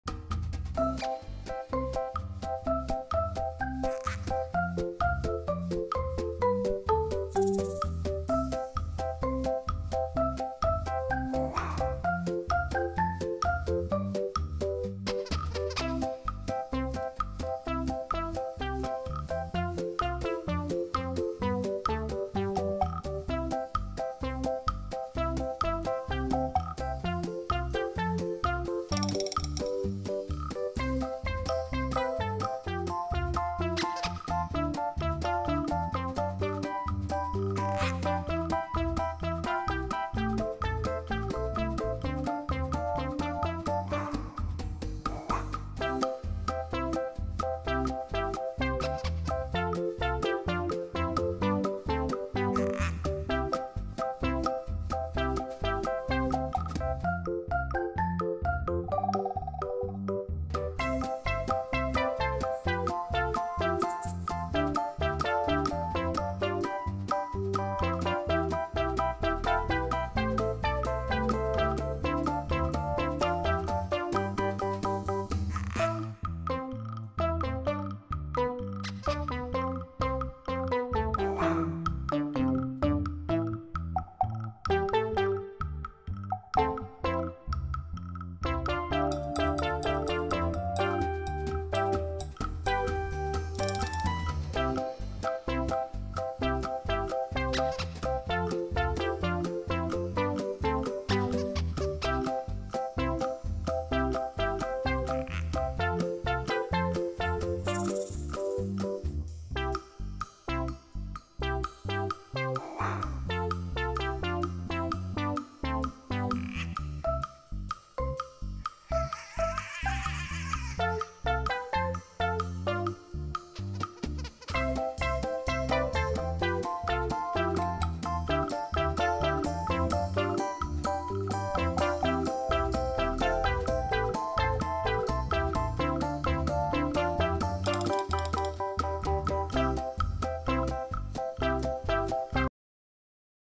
Golden Retriever Takes Revenge On Kitten for Not Letting Him Sleep
Golden Retriever Takes Revenge On Kitten for Not Letting Him Sleep#goldenretriever